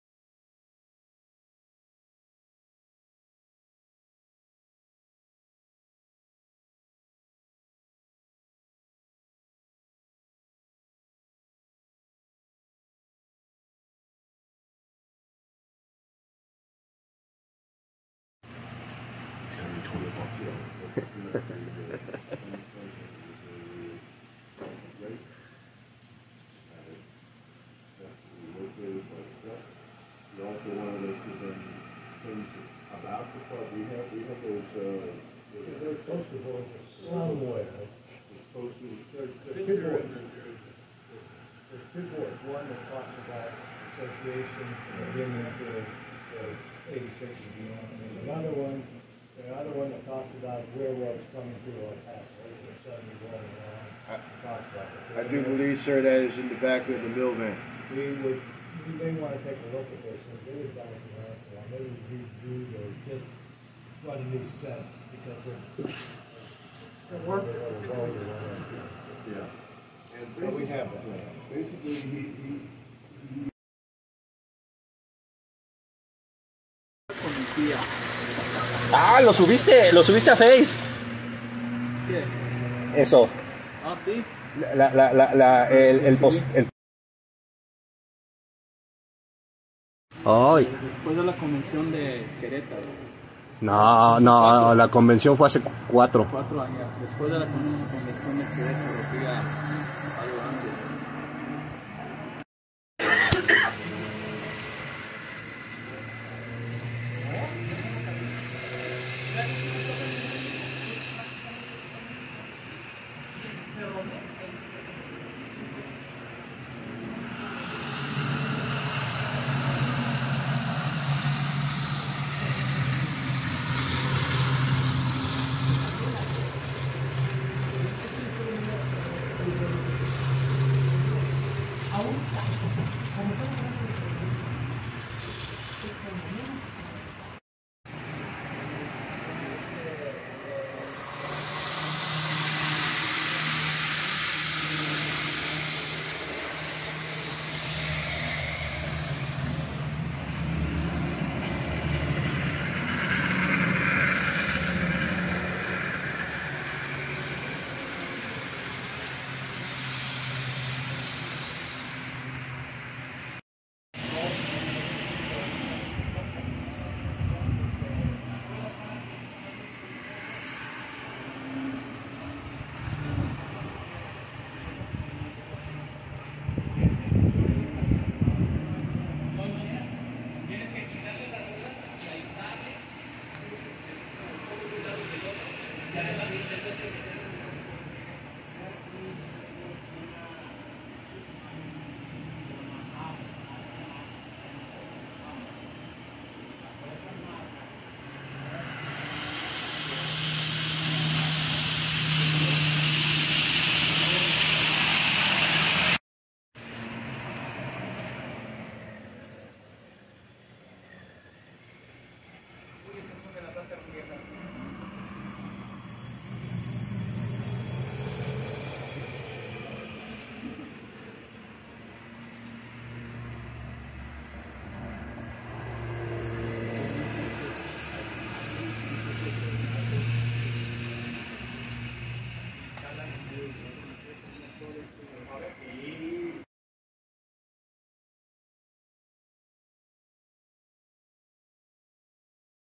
El Paso Model Railroad Club 19-08-2021
Segunda visita de este viaje que realize a El Paso Tx en agosto del 2021.